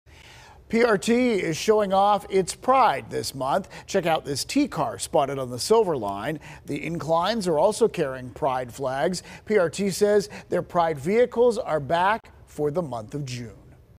PRT Pride vehicles back for June
A Pride-themed train was spotted on the Silver Line.